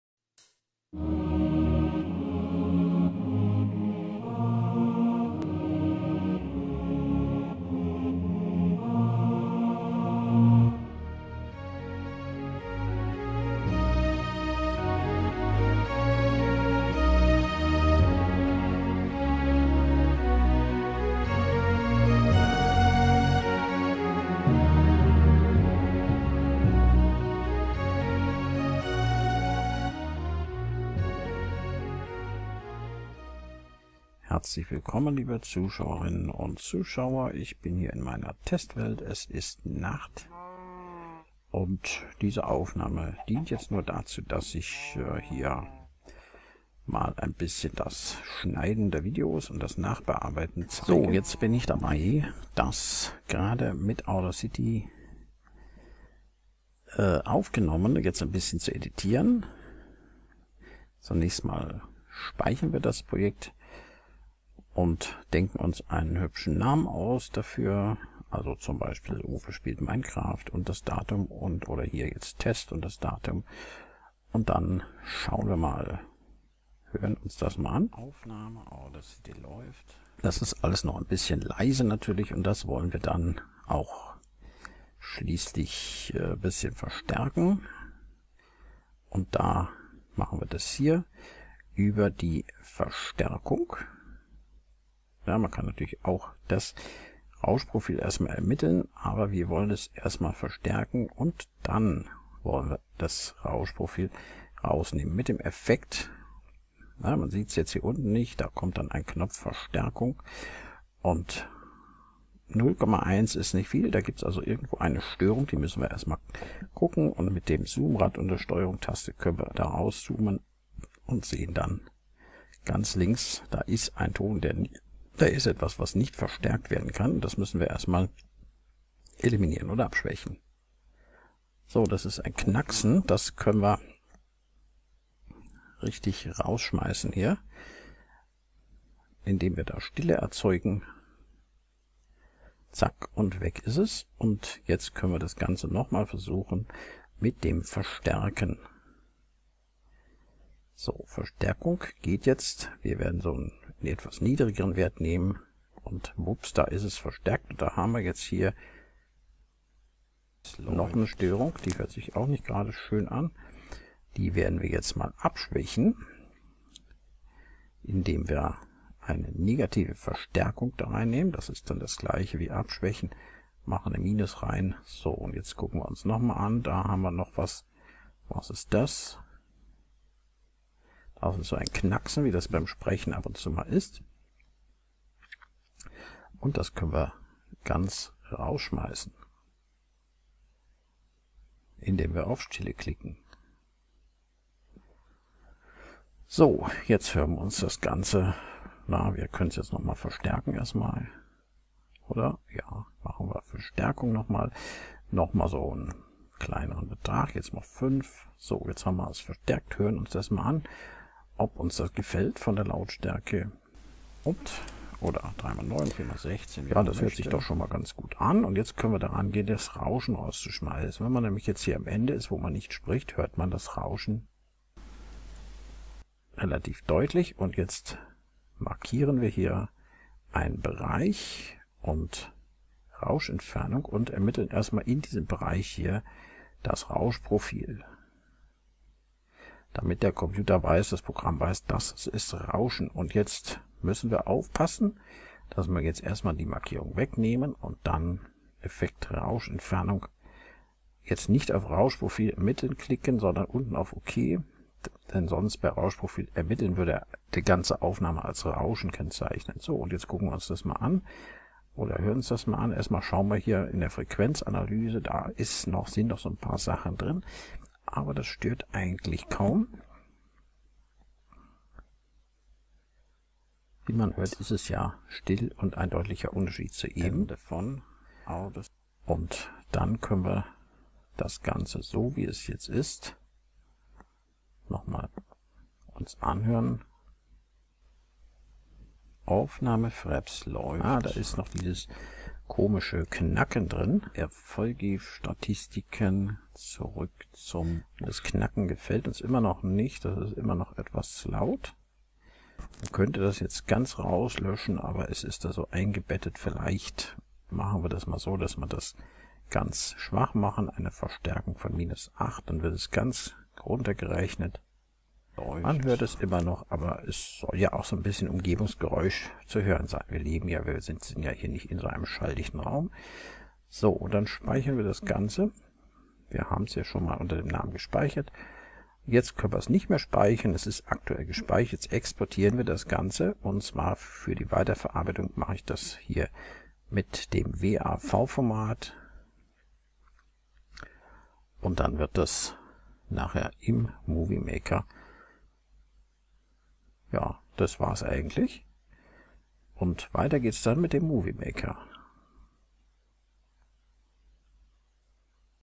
Tutorial (3) | Kommentar mit Audacity bearbeiten
xxxxxxxxxxxxxxxxxxxxxxxxxxx
Wie mache ich meinen Kommentar lauter? Wie eliminiere ich das Rauschen? Wie reduziere oder lösche ich störende Geräusche?